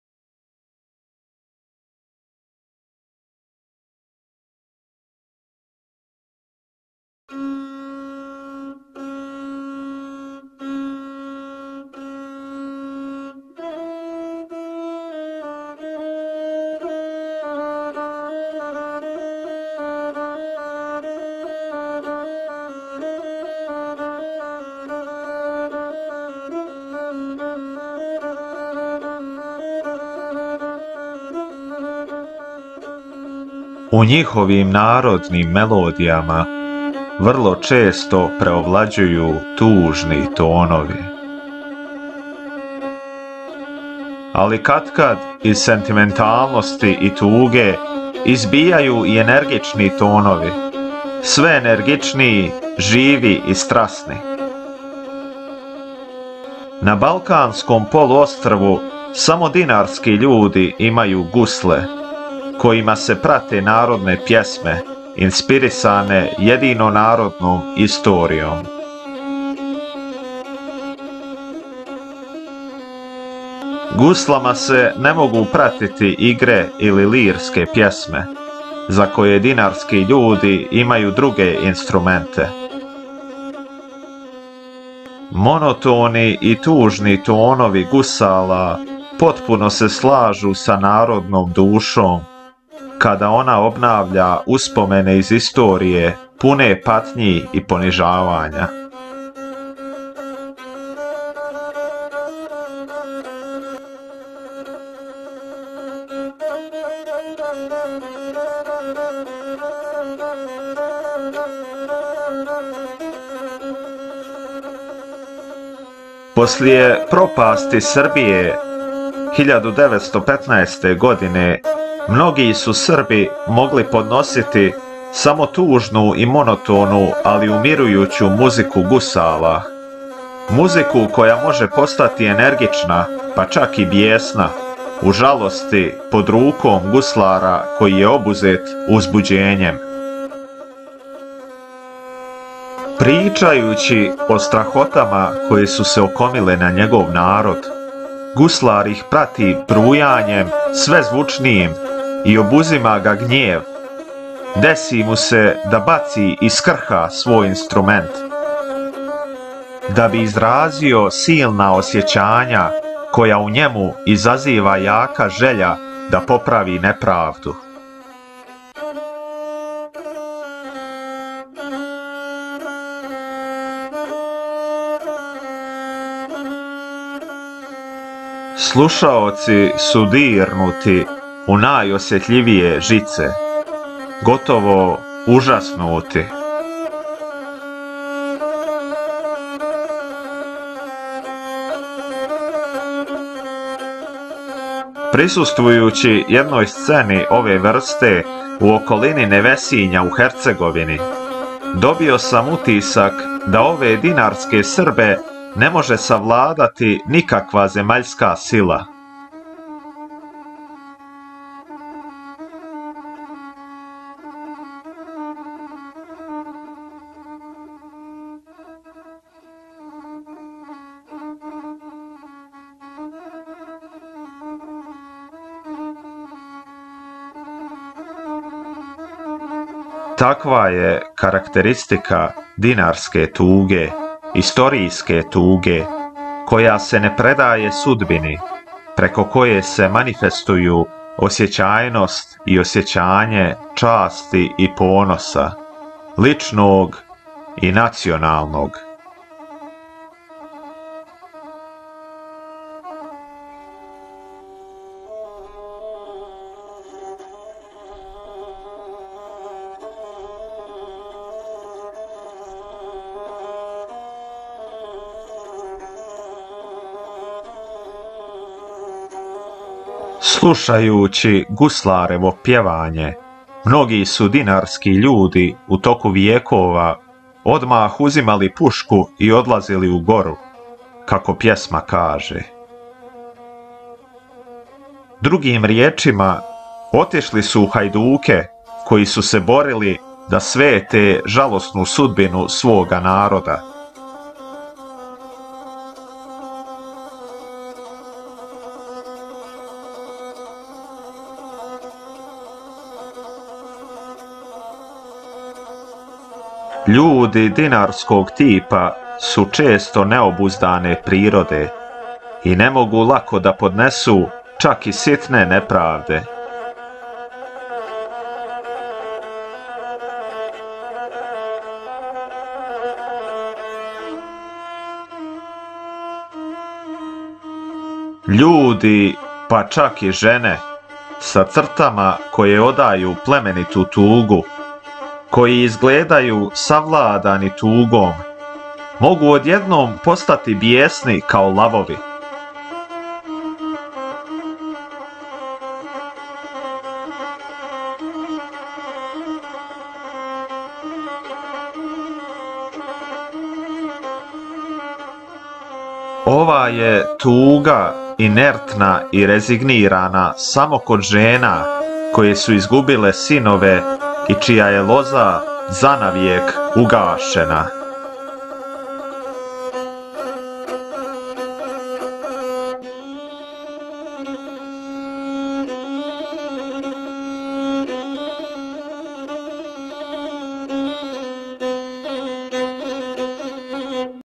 Dinaric People - Part 7: Dinaric Tone / Седми дио: Динарски Тон
Part seven of the detailed ethnographic description of the Dinaric peoples given by the ethnologist and geographer Jovan Cvijić (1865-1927) in his book titled Balkan Peninsula.

Седми дио детаљног етнографског описа динарског народа од стране српског етнолога и географа Јована Цвијића (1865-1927) из његове књиге Балканско полуострво и Јужнословенске земље.